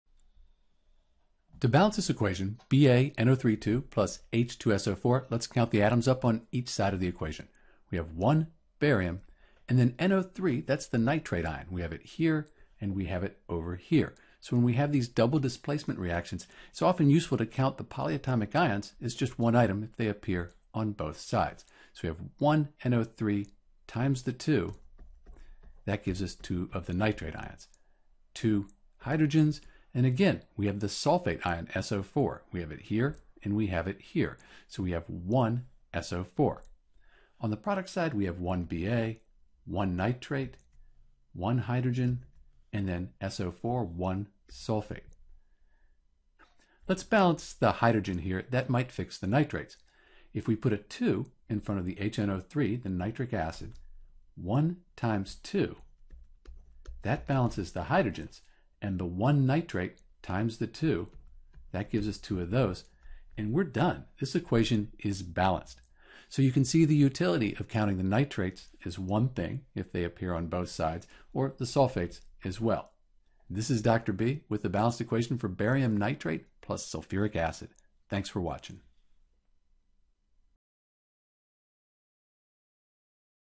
How to Balance Ba(NO3)2 + H2SO4 = BaSO4 + HNO3 (Barium nitrate + Sulfuric acid)
In this video we'll balance the equation Ba(NO3)2 + H2SO4 = BaSO4 + HNO3 and provide the correct coefficients for each compound.

To balance Ba(NO3)2 + H2SO4 = BaSO4 + HNO3 you'll need to be sure to count all of atoms on each side of the chemical equation.

Once you know how many of each type of atom you can only change the coefficients (the numbers in front of atoms or compounds) to balance the equation for Barium nitrate + Sulfuric acid.